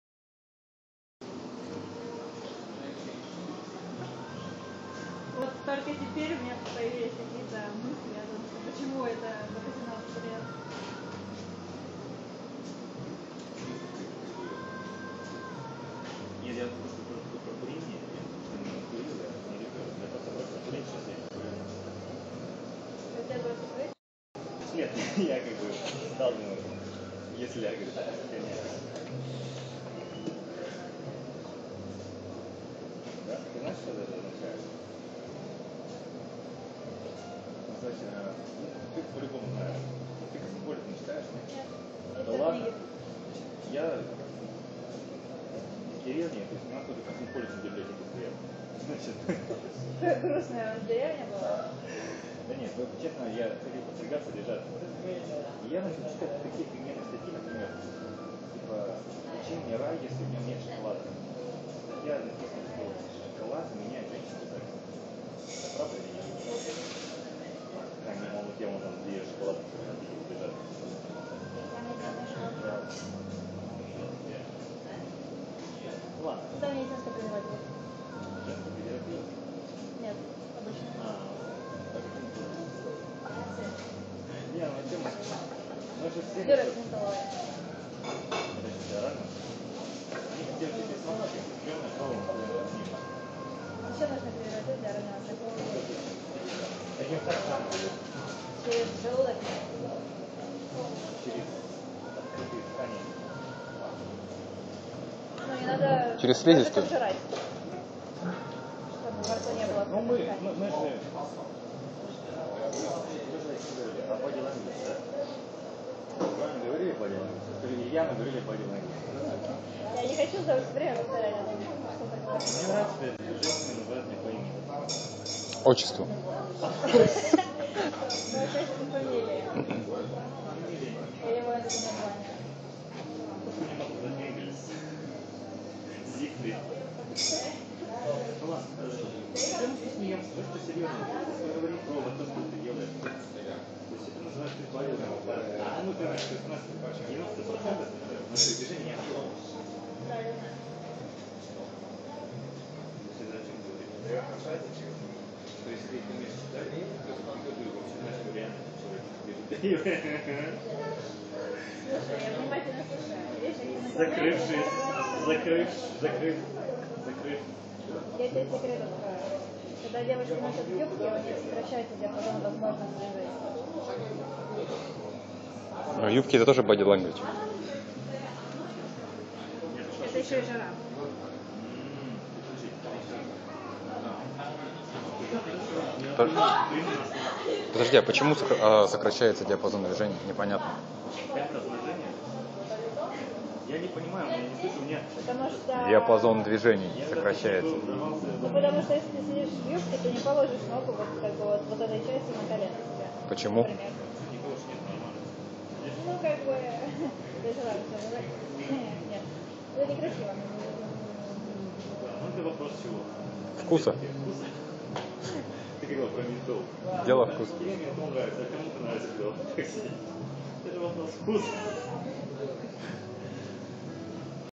My Toronto life: Talking to a Russian friend/ Беседа с русской девушкой в Торонто, Канада Part 2
This video was filmed as part of the Healthy Self-image practical exercise based on the book by Dr. Maltz Psychocybernetics. He developed techniques for improving and managing self-image—visualization, mental rehearsal, relaxation—which have informed and inspired countless motivational gurus, sports psychologists, and self-help practitioners for more than fifty years